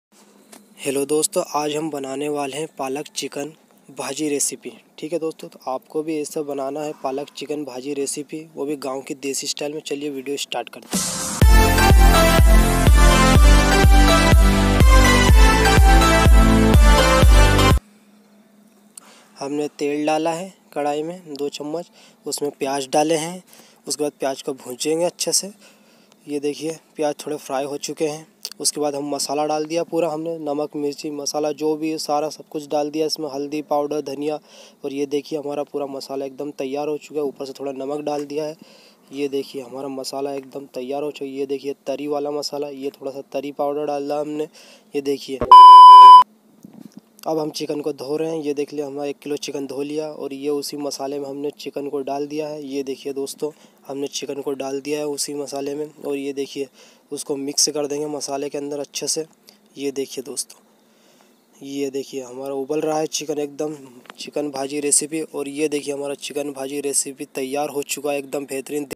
Village Style Chicken Gravy | Simple and Instant Chicken बनाए और इस तरहा का चिकन आपने कभी नही खाया
How to make recipe :

1kg chicken

1-5 people recipe :

Ingredients :

2 spun oil
2 onion
2 tomato
1-2 hara dhaniya
1 tsp red chilli
1 tsp haldi powder
1tsp ginger garlic paste
1 tsp salt